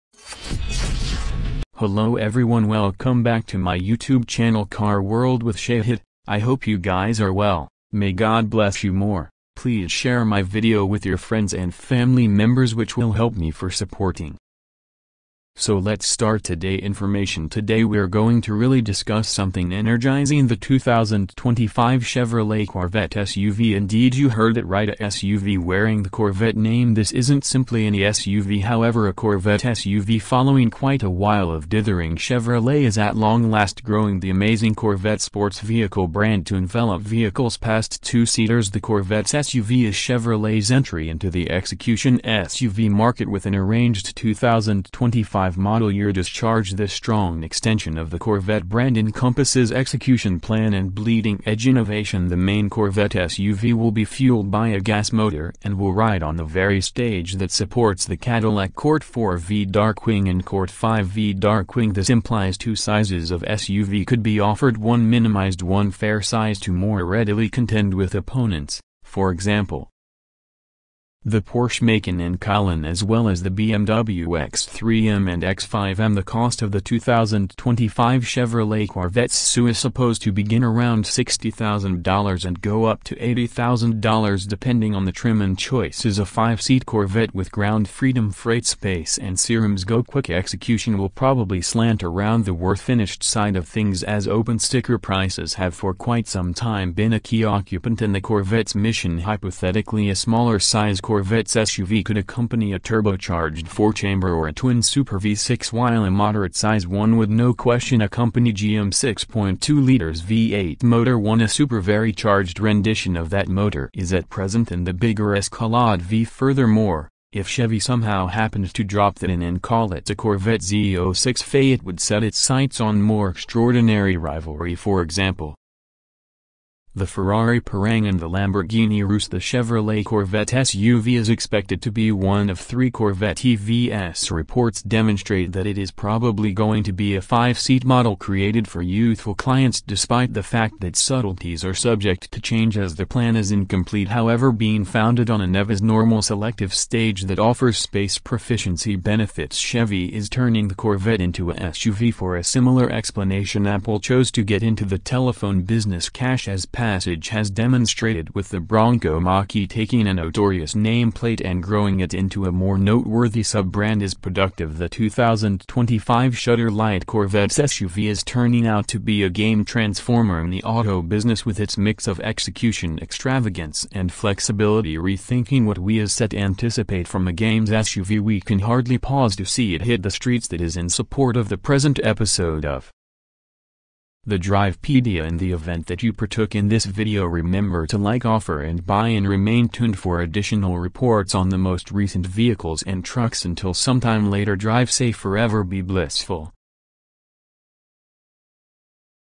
"Chevrolet Unveils Bold New Addition: The 2025 Corvette SUV!"
Chevrolet has set the automotive world abuzz with its latest announcement: the 2025 Corvette SUV. This bold new addition combines the iconic style and performance of the Corvette with the practicality and versatility of an SUV. Featuring sleek lines, cutting-edge technology, and powerful performance capabilities, the Corvette SUV promises to deliver an exhilarating driving experience on and off the road. With this innovative model, Chevrolet is pushing boundaries and redefining what it means to drive a Corvette,